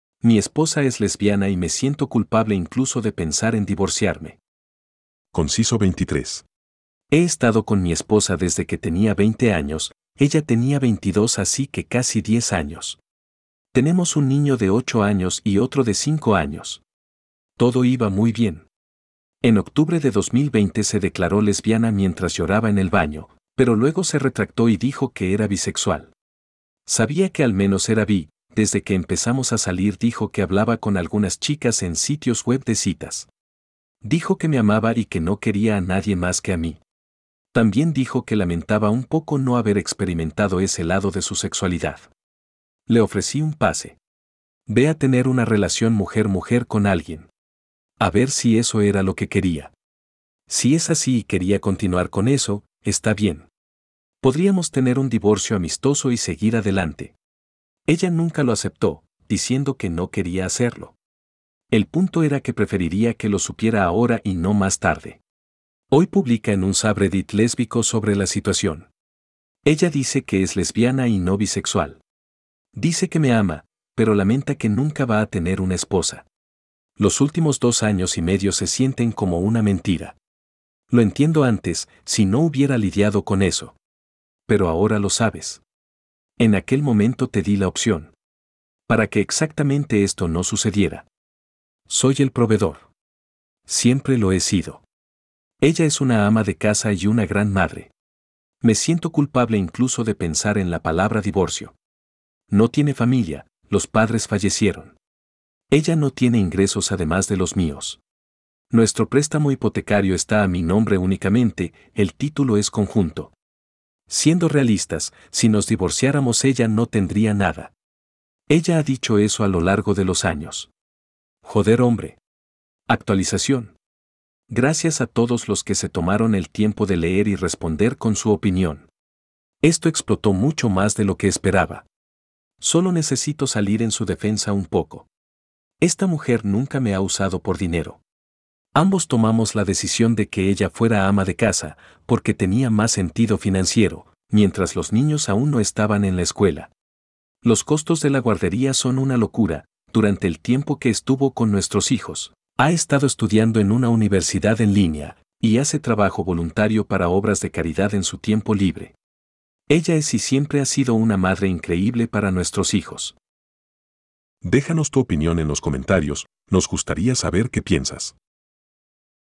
Mi esposa es lesbiana y me siento culpable - Con Actualización - Reddit Español
Mi esposa es lesbiana y me siento culpable incluso de pensar en divorciarme.
Con Actualización

Créditos:
Foto thumbnail